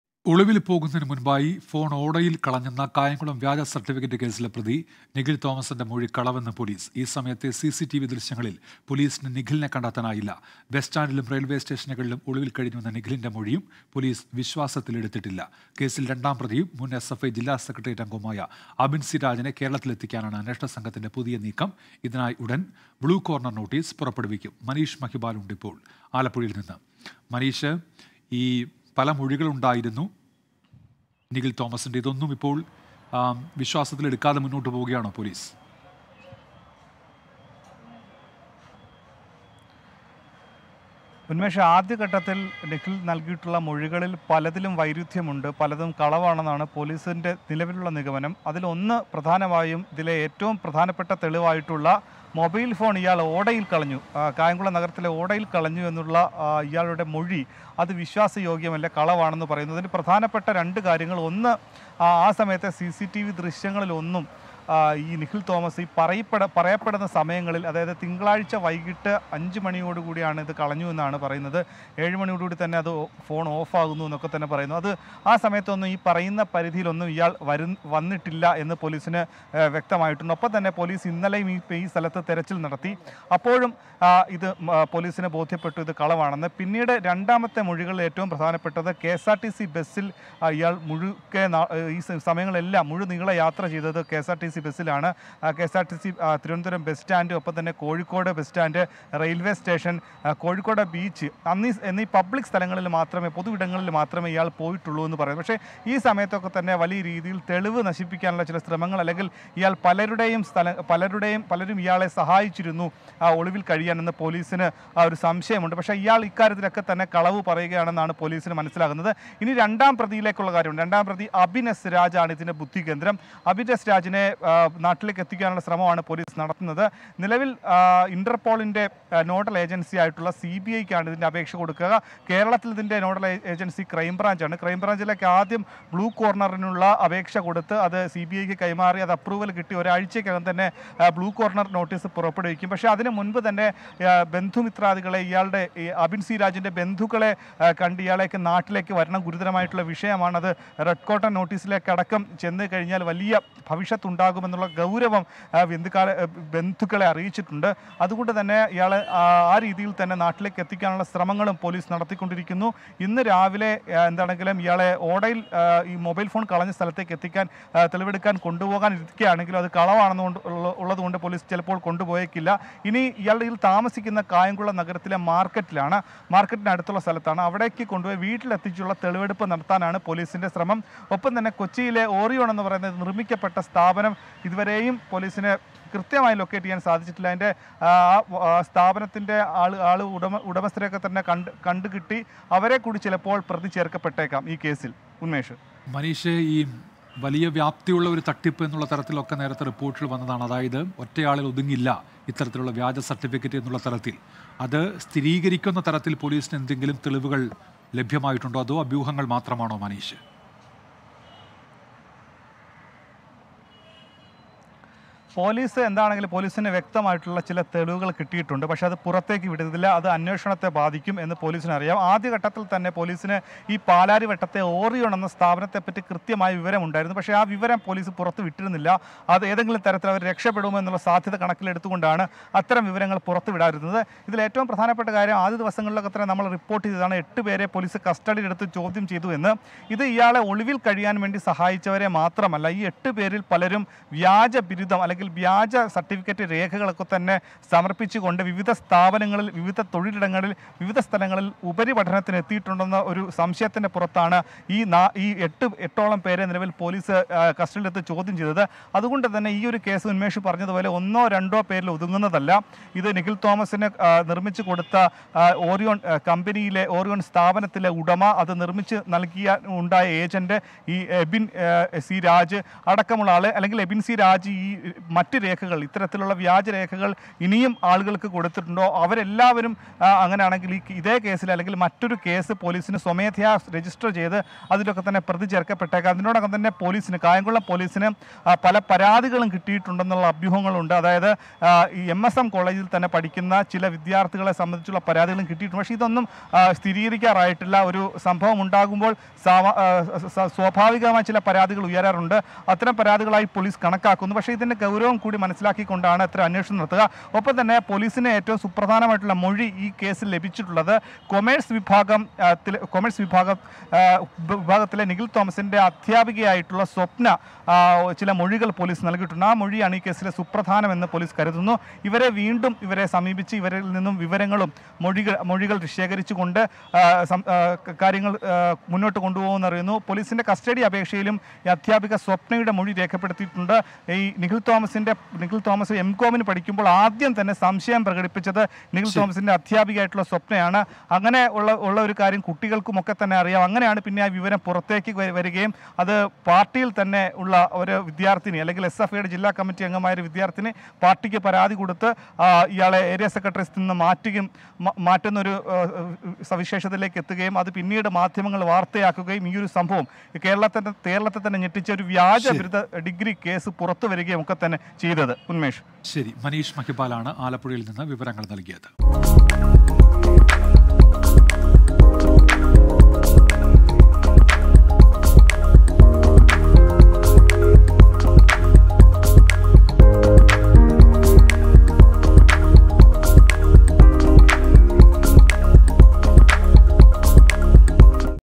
ഒളിവിൽ പോകുന്നതിന് മുമ്പായി ഫോൺ ഓടയിൽ കളഞ്ഞെന്ന നിഖിൽ തോമസിന്റെ മൊഴി കളവെന്ന് പൊലീസ്
ഒളിവിൽ പോകുന്നതിന് മുമ്പായി ഫോൺ ഓടയിൽ കളഞ്ഞെന്ന കായംകുളം വ്യാജ സർട്ടിഫിക്കറ്റ് കേസിലെ പ്രതി നിഖിൽ തോമസിന്റെ മൊഴി കളവെന്ന് പൊലീസ്

Kayamkulam fake certificate case accused Nikhil Thomas's statement that he had thrown his phone in the drainage before going on the run, police said.